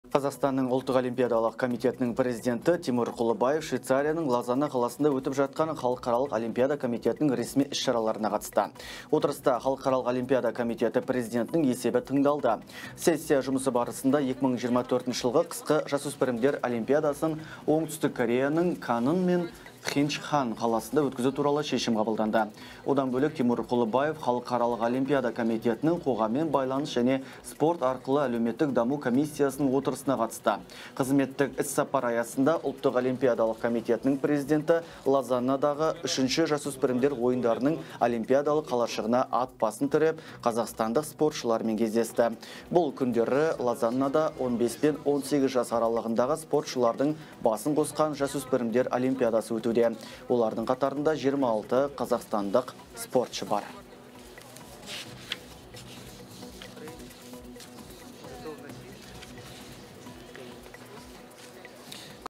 ТИМУР ҚҰЛЫБАЕВ ҚАЗАҚСТАН СПОРТШЫЛАРЫМЕН КЕЗДЕСТІ
Қазақстанның Ұлттық Олимпиалалық Комитетінің президенті Тимур Құлыбаев Швейцарияның Лозанна қаласында өтіп жатқан Халықаралық Олимпиада комитетінің ресми іс-шараларына қатысты. Отырыста Халықаралық Олимпиада Комитеті президентінің есебі тыңдалды. Сессия жұмысы барысында 2024 жылғы қысқы жасөспірімдер Олимпиадасын Оңтүстік Кореяның Канын мен Пхенчхан қаласында өткізу туралы шешім қабылданды. Одан бөлек, Тимур Құлыбаев Халықаралық Олимпиада Комтетінің қоғаммен байланыс және спорт арқылы әлеуметтік даму Комиссиясының отырысына қатысты. Қызметтік іссапар аясында Ұлттық Олимпиадалық Комитетінің президенті Лозаннадағы үшінші жасөспірімдер ойындарының олимпиадалық қалашығына ат басын тіреп, қазақстандық спортшылармен кездесті. Бұл күндері Лозаннада 15-18 жас аралығындағы спортшылардың басын қосқан жасөспірімдер Олимпиадасы өтуде. Олардың қатарында 26 қазақстандық спортшы бар.